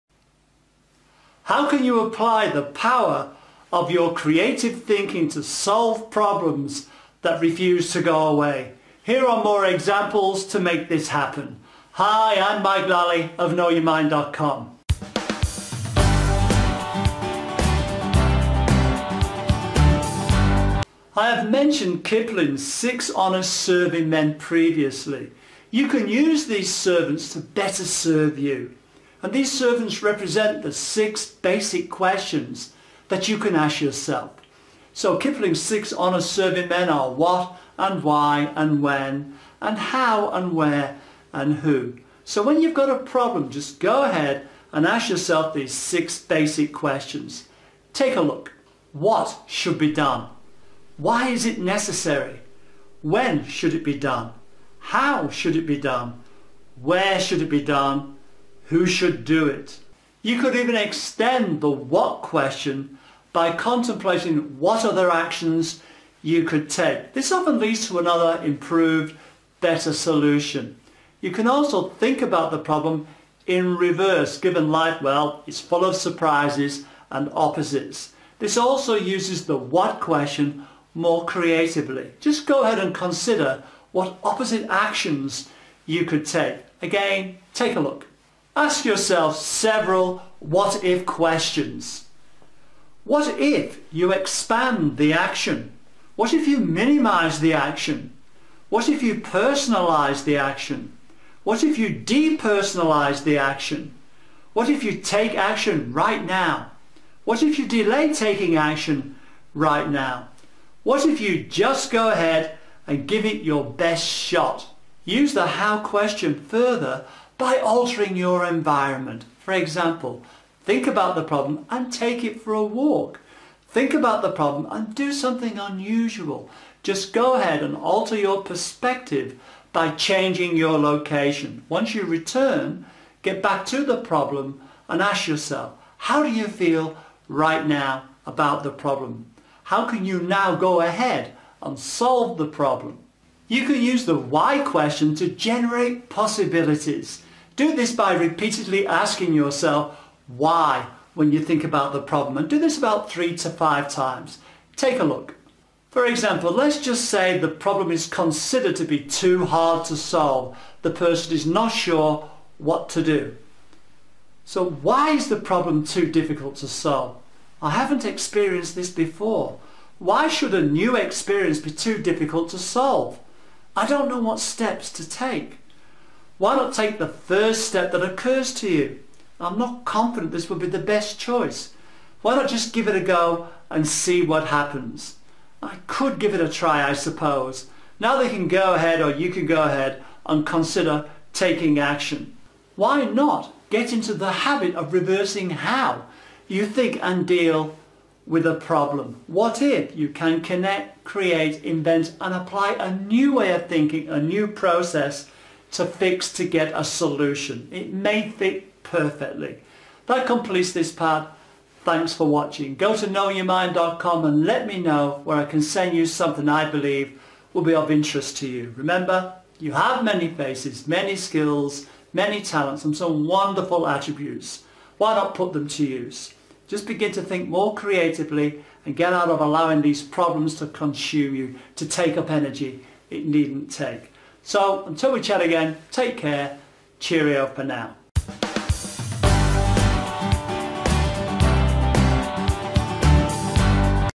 How to Think Creatively PART FOUR
How can you use the power of your creative mind to make the changes you want to get the benefits that you would love to have?

Your brain is like a well-oiled machine. It already knows how to be creative. Having the techniques to improve your creative thinking can make a huge difference to your results.

Creative thinking and accelerated learning go hand in hand. Given you have the ability to learn anything you want, so why not harness the creativity of your thinking to learn something new?

Learning and creativity strategies and techniques are the key.
Whether it is convergent thinking, divergent thinking, critical thinking, creative thinking, or generating ideas, stimulating your creativity will help you to come up with fresh, new ways of doing things.

Understanding creativity techniques will allow you to solve problems, and take advantage of opportunities. We all have natural creativity, though many people do not know the simple techniques that can be used to accelerate and strengthen this process.

The ability to overcome the prejudices of logic by examining a variety of creative thinking techniques is a wonderful gift.

New concepts and thinking strategies will quickly emerge to inspire you.
Go ahead and "see what others do not see", "think what others are not thinking and develop an "opportunity mindset".

Brainstorming, and stimulating your creativity, can make a complete difference to your results in life.